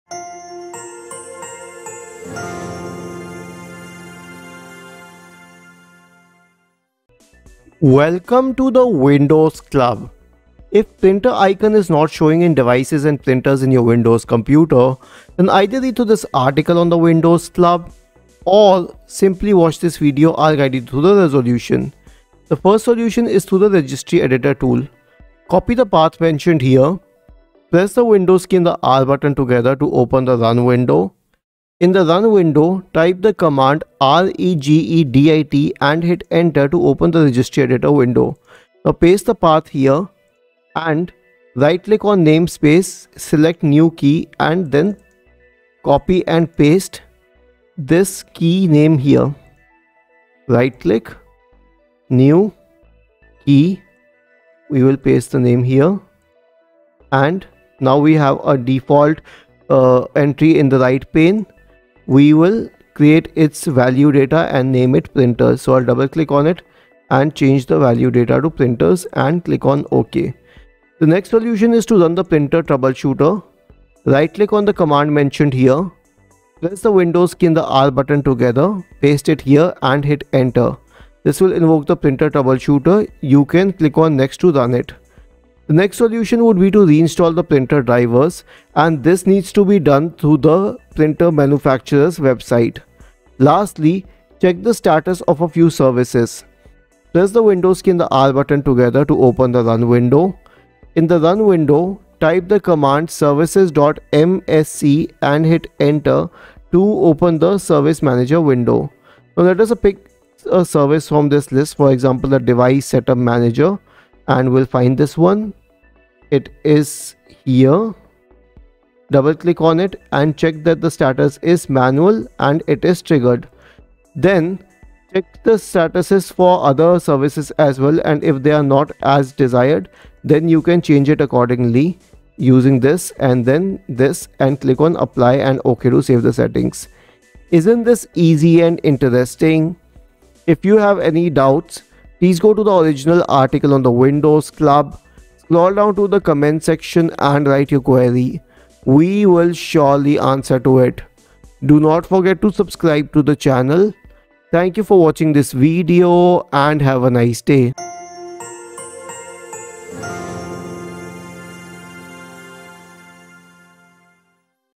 Printer icon not showing in Devices and Printers in Windows 11/10
If you find that the Printer icon is not showing in your Desktop, Control Panel, Devices and Printers, then you may have to tweak the Windows Registry. You will have to follow the same procedure as you did to list the Printers using the same printer driver separately.

Here are a few fixes and suggestions:

1] Check Registry key

2] Run Printer Troubleshooter

3] Update Printer Drivers

4] Reinstall Printer driver

5] Check Services status

Timecodes:
0:00 Intro
0:22 Registry Editor fix
1:12 Run the Printer troubleshooter
1:27 Reinstall the rpinter drivers
1:37 Check status of a few services